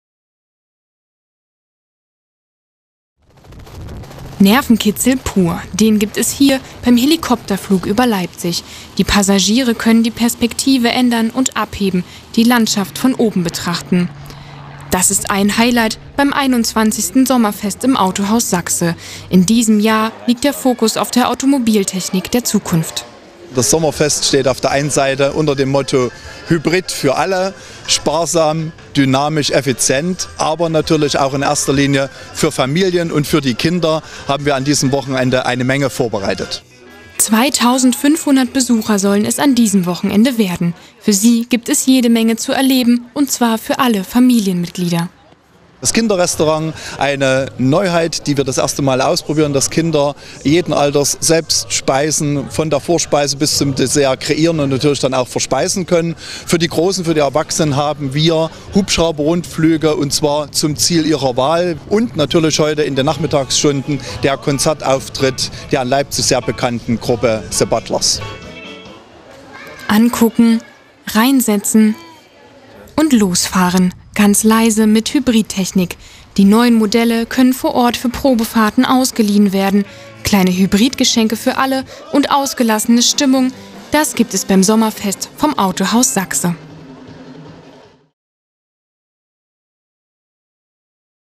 Sommerfest im Autohaus Saxe Leipzig
Leipzig, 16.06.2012 Helikopterflüge, Live-Musik und Probefahrten: Im Autohaus Saxe feiern ca. 2500 Besucher das 21. Sommerfest.